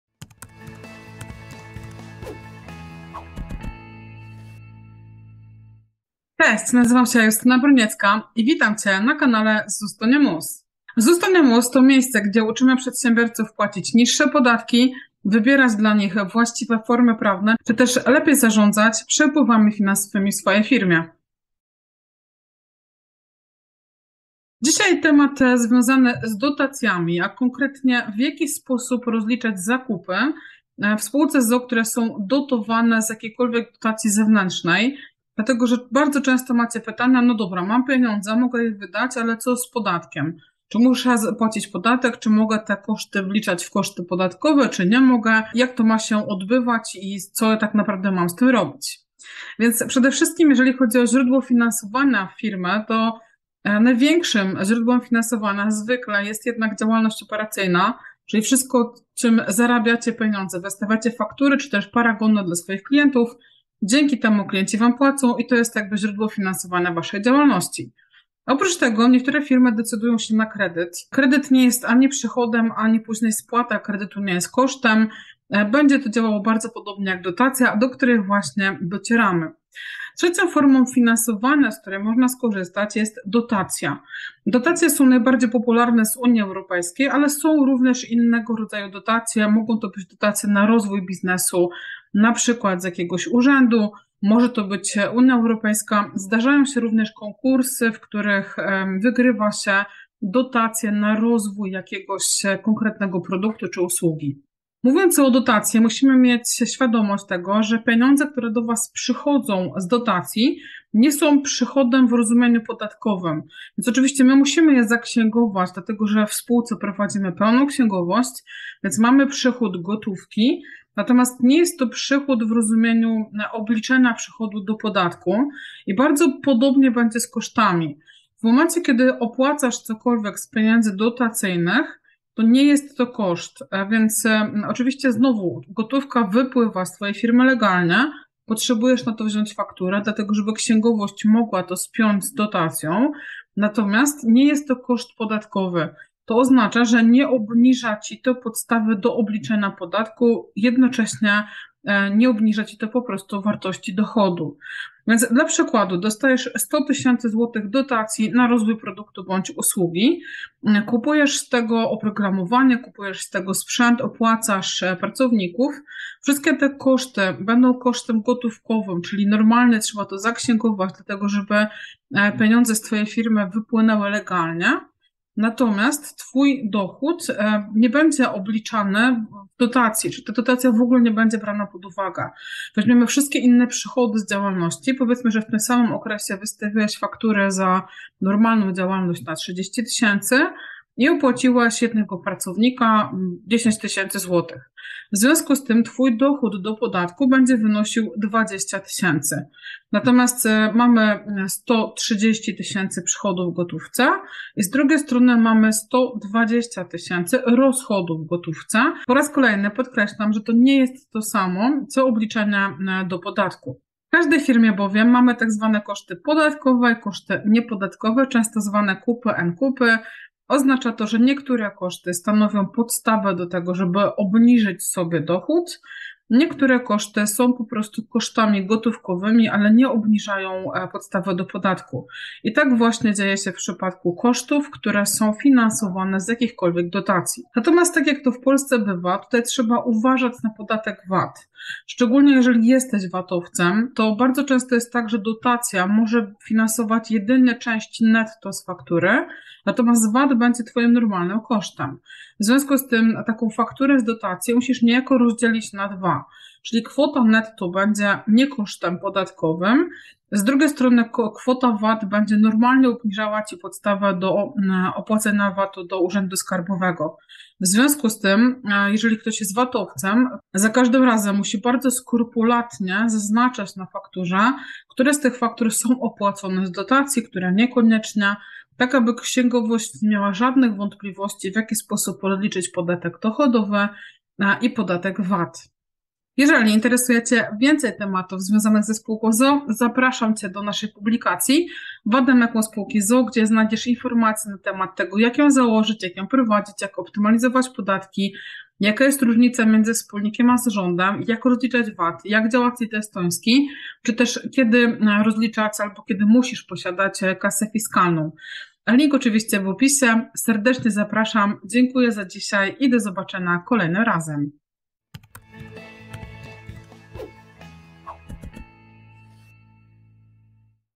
Jak rozliczać zakupy z dotacji w spółce z o.o.?
Różnego rodzaju dotacje 💰 do działalności są coraz bardziej popularne. Są to dotacje nie tylko na założenie działalności, ale również na jej rozwój. W dzisiejszym filmie poruszamy kwestię rozliczania dotacji, odpowiadamy na pytanie, czy możemy wrzucić je w koszty podatkowe w spółce z o.o., co z VAT-em itd. 🤔

Jeżeli interesuje Cię więcej informacji na temat prowadzenia spółki z o.o., zapraszam Cię do zakupu naszego poradnika Vademecum Sp. z o.o.